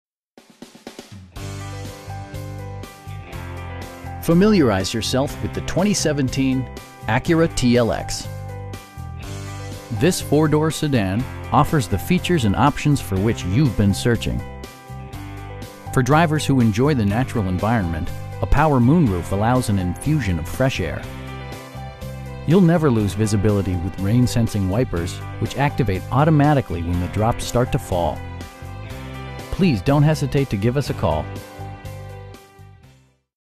2017 Acura TLX 3.5 V-6 9-AT P-AWS with Advance Package
13553 Research Blvd.

Familiarize yourself with the 2017 Acura TLX. This 4 door sedan offers the features and options for which you've been searching. For drivers who enjoy the natural environment, a power moon roof allows an infusion of fresh air. You'll never lose visibility with rain sensing wipers, which activate automatically when the drops start to fall. Our aim is to provide our customers with the best prices and service at all times. Please don't hesitate to give us a call.